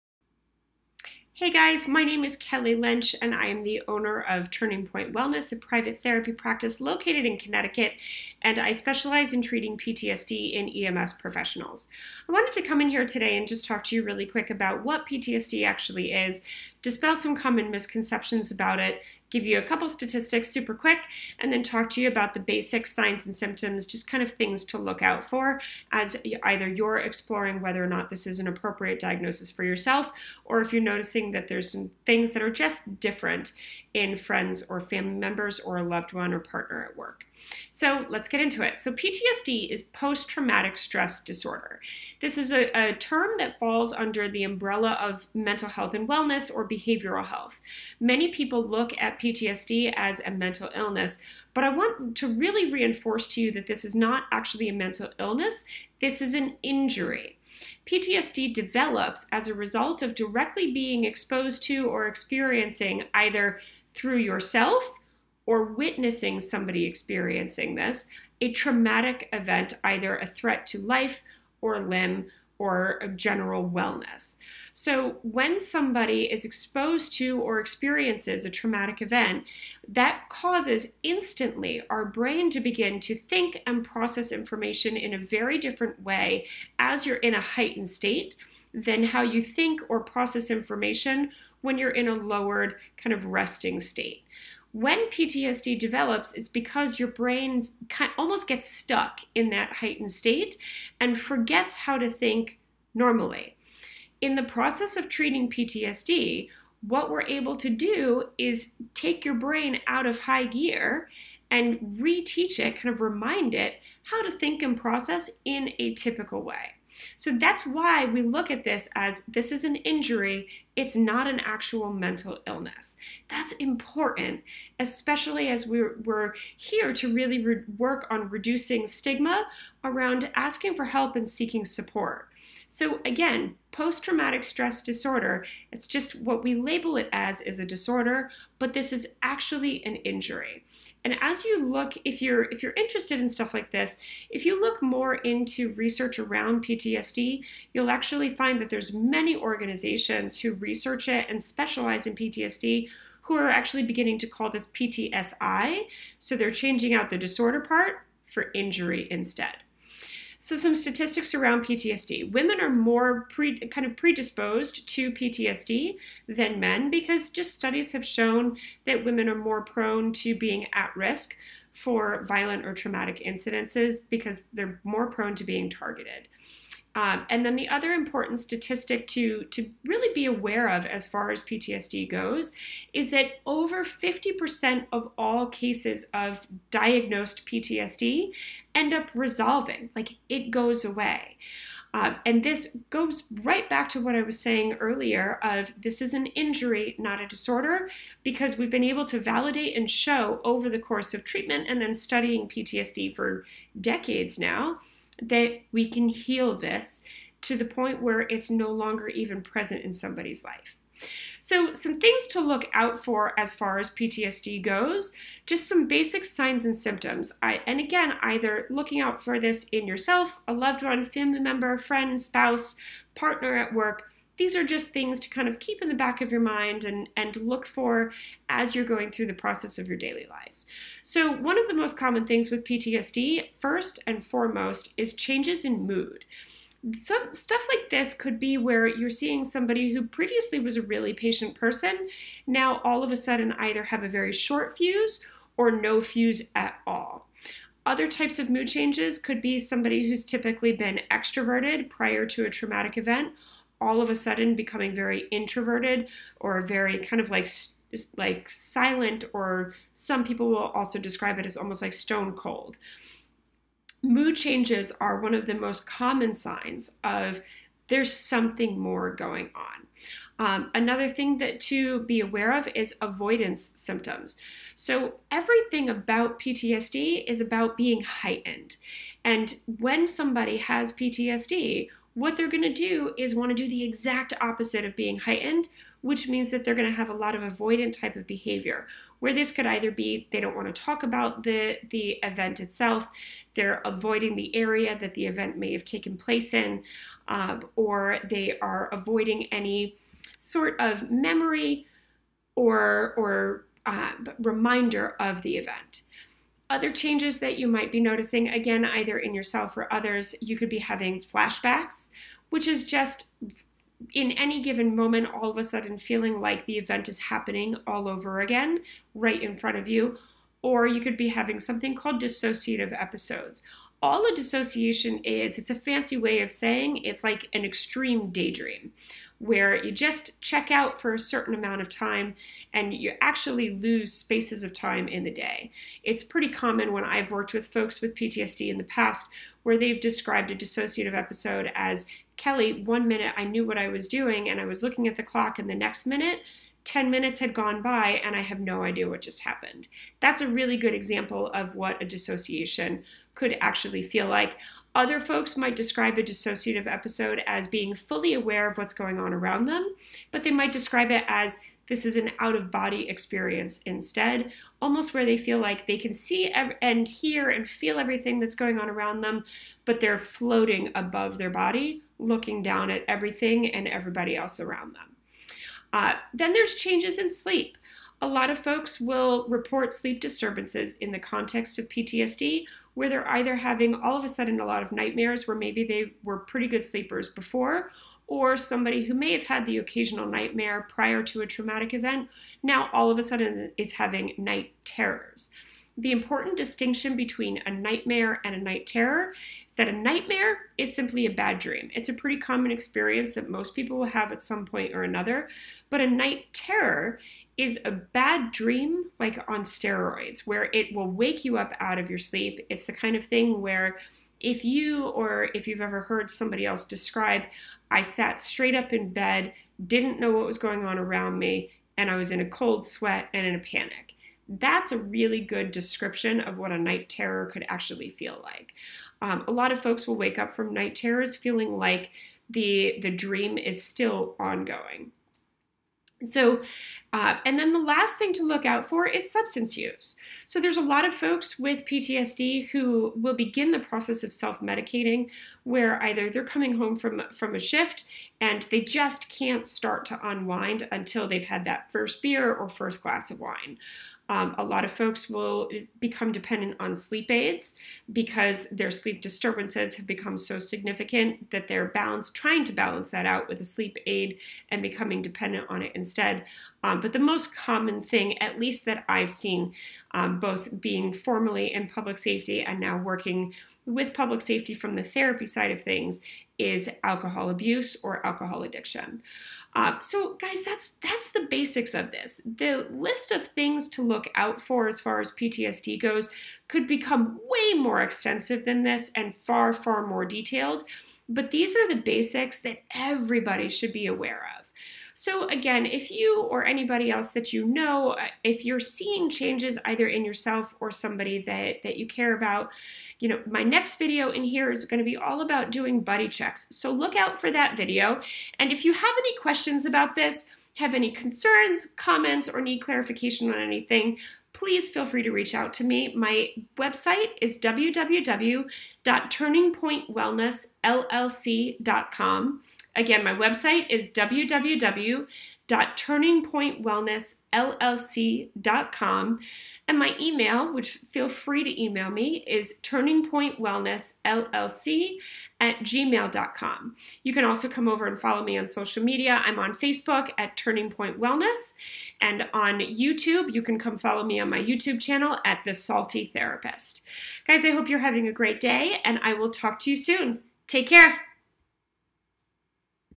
CAPE and Turning Point Wellness - PTSI
CAPE partners with Licensed Clinical Social Worker Kelly Lynch from Turning Point Wellness LLC to help bring knowledge, support, and resources to First Responders.